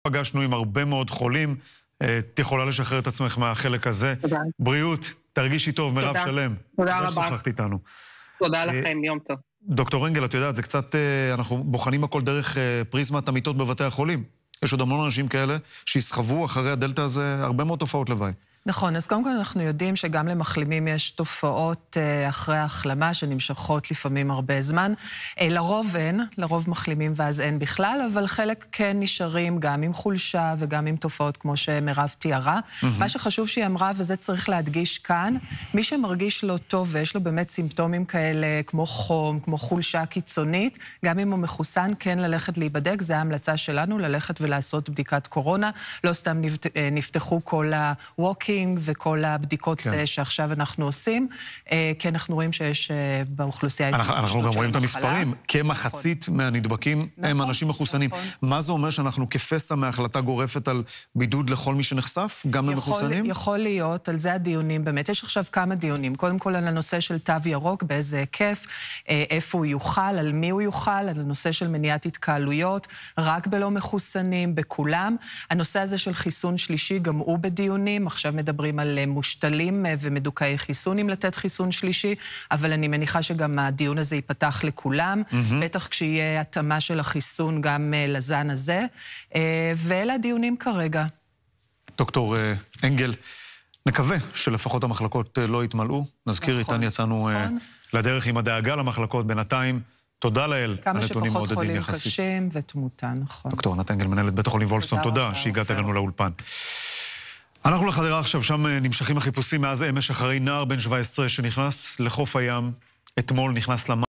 הקורונה בישראל: מה צפוי לנו בחודשים הקרובים?
מנכ"לית בית החולים וולפסון, ד"ר ענת אנגל, עונה על כל השאלות המסקרנות בראיון לחדשות 12